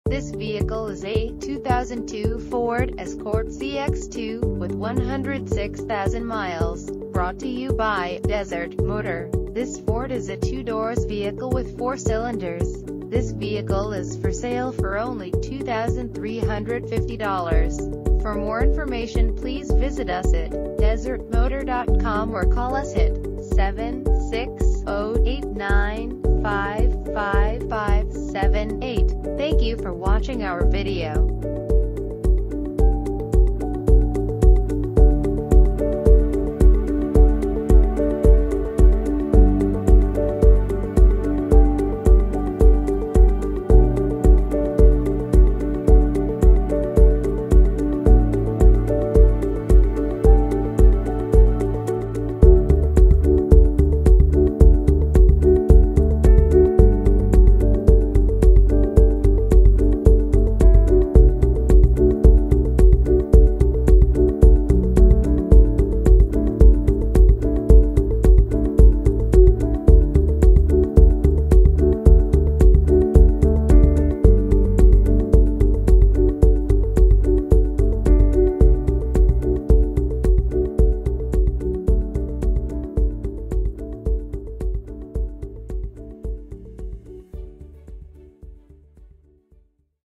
2002 Ford Escort ZX2 - DESERT MOTOR
DESERT MOTOR
74990 Joni Dr

2002 Ford  Escort ZX2
WAS $ 2,700 NOW $ 2,350

Clean title, Extended Warranty Available, 106,000 miles, Automatic, Oxford White exterior, Dark Charcoal interior, Coupe, FWD: Front Wheel Drive, 2 doors, 4 cylinders, Gasoline, MPG City 28 and 34 in Hwy.